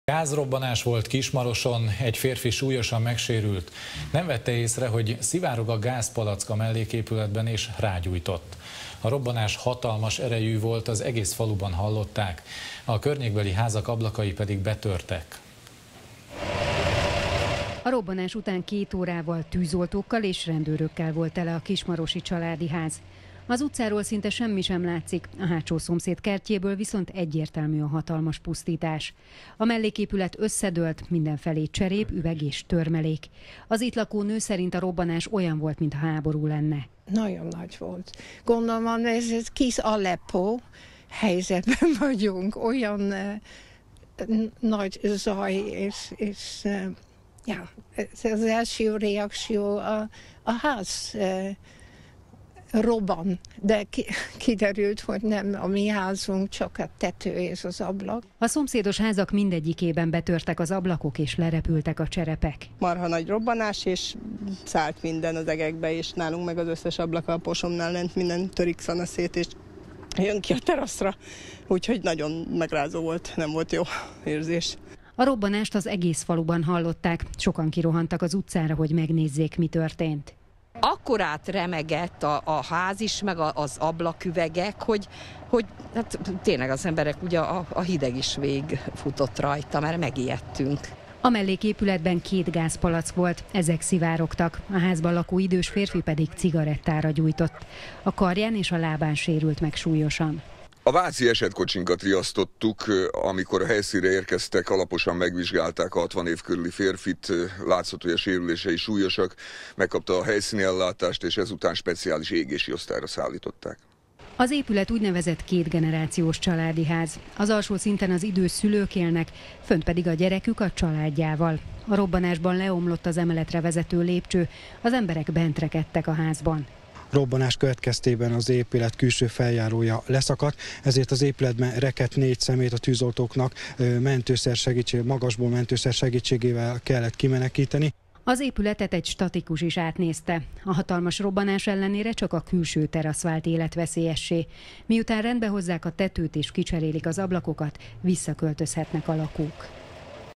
Óriási robbanás volt egy kismarosi családi házban
Gázrobbanás volt Kismaroson. Egy férfi súlyosan megsérült.

A legfrissebb hírekért, videókért, cikkekért látogasson el az atv.hu oldalára. Facebook